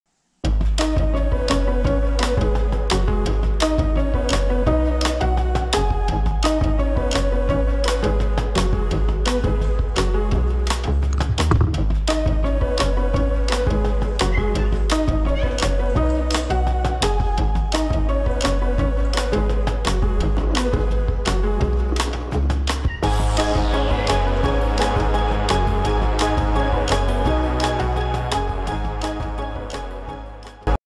Пол минуты умиления. Играю с кошкой
Кошка приносит резинку в зубах